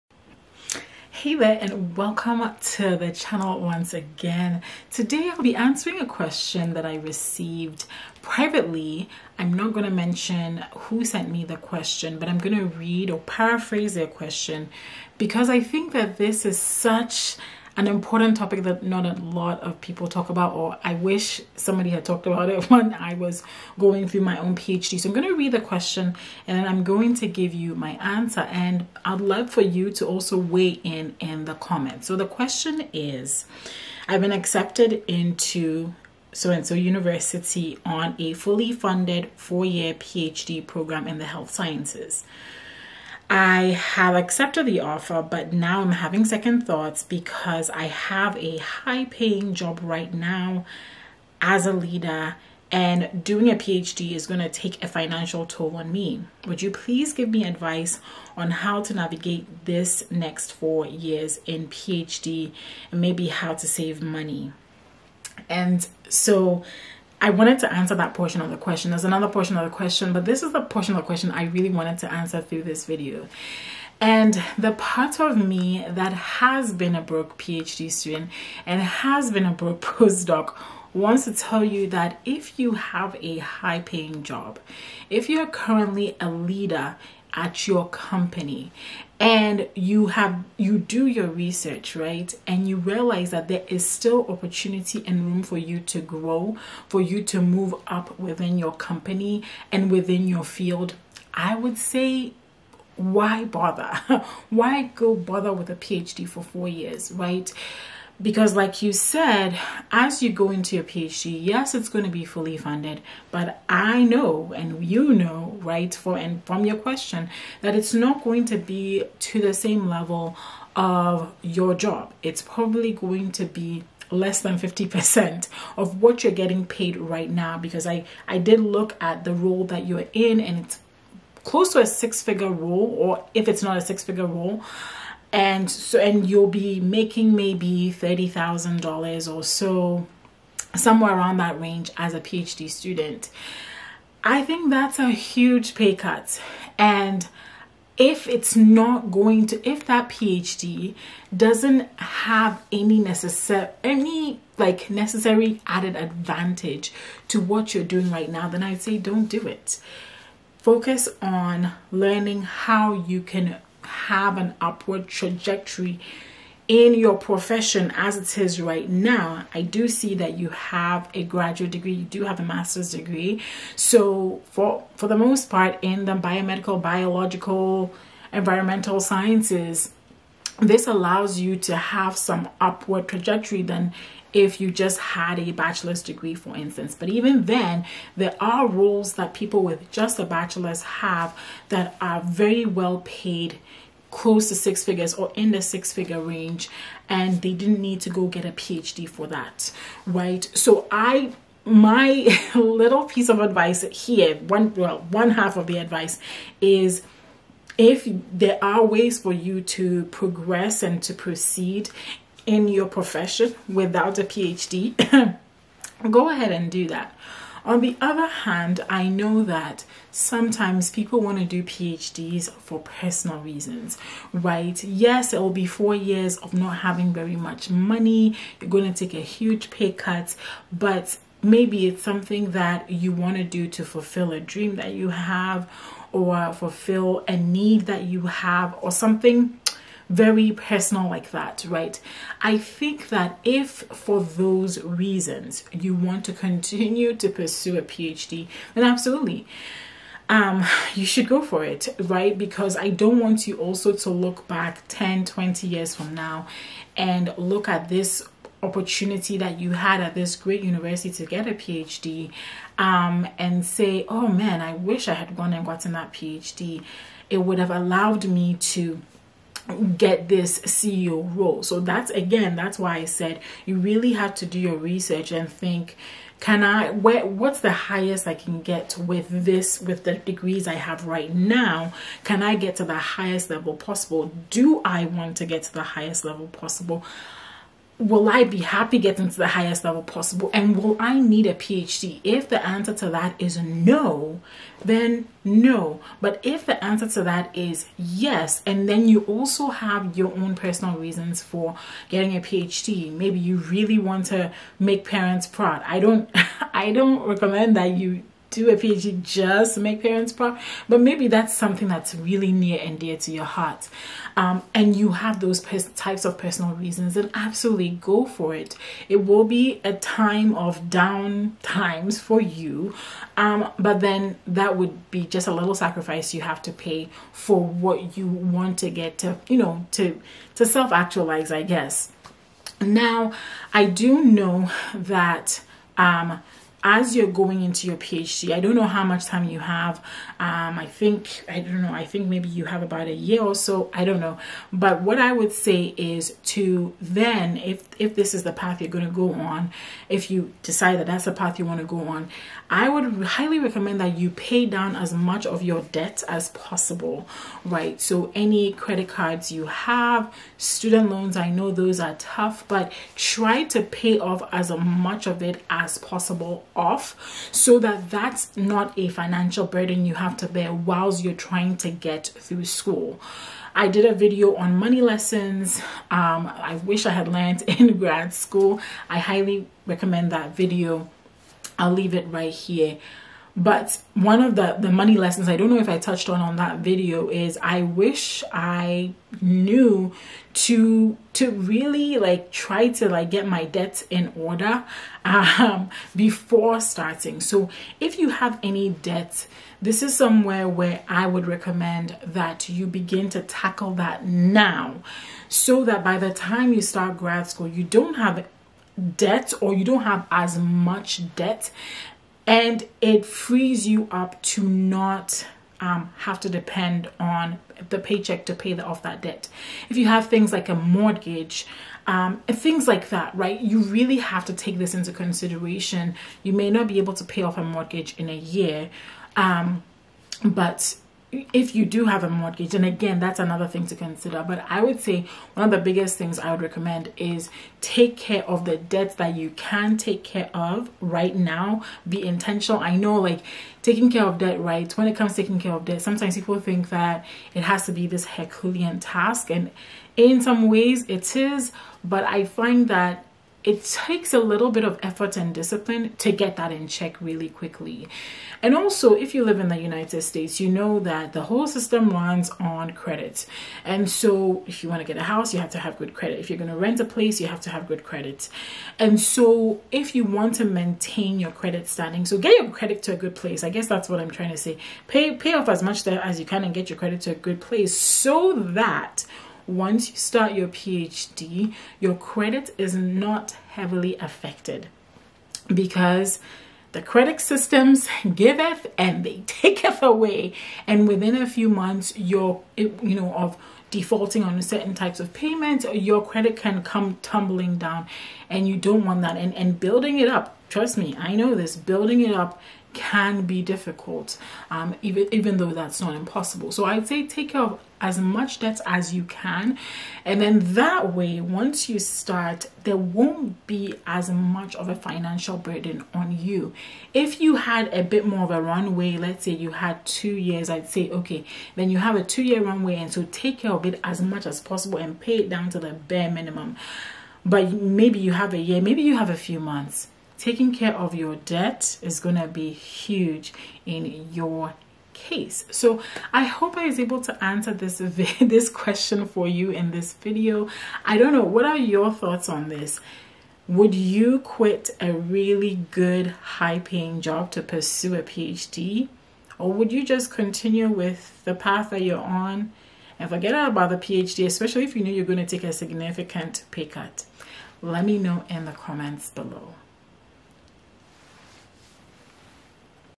Should I quit my HIGH-PAYING JOB for a PhD?
Hey there biomeds! Today, I am answering a question I got on LinkedIn around quitting a high-paying job for a fully-funded PhD. Here is my response to that!